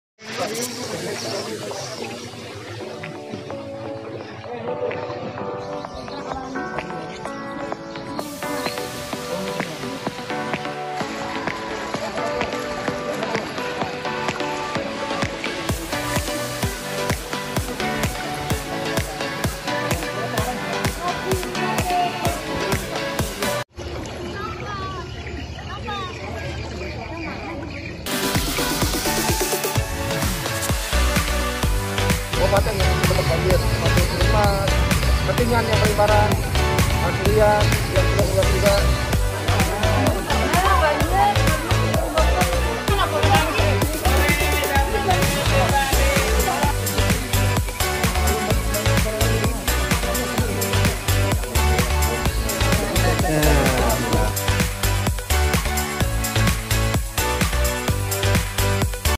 GUBERNUR KALTENG TURUN LANGSUNG PANTAU BANJIR
Semoga dengan adanya kedatangan Pa guberbur Sugianto Sabran dan para instansi yang sudah membantu dilokasi banjir bisa mengurangi beban para masyarakat yang sedang dilanda musibah semoga banjir ini cepat surut dan kita semua selalu diberikan kesehatan aamiin